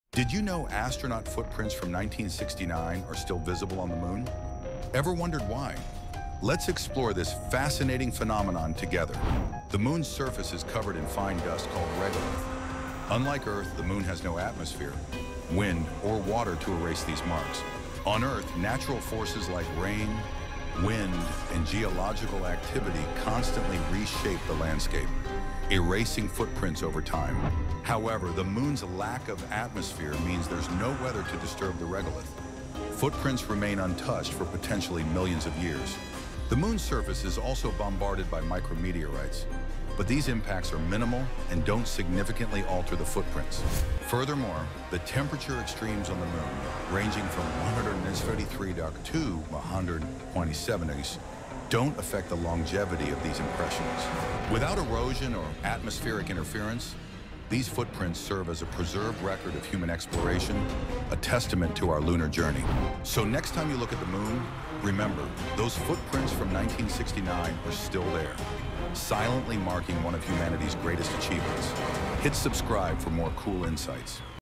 Astronaut Footprints Still on the Moon After 56 Years ??
Did you know that the footprints left by astronauts in 1969 are still visible on the Moon? 🌕 Join us as we explore why these marks remain untouched for millions of years! With no atmosphere, wind, or water to erase them, the Moon preserves our lunar legacy. Discover how the unique conditions of the Moon make these footprints a lasting testament to human exploration. 🚀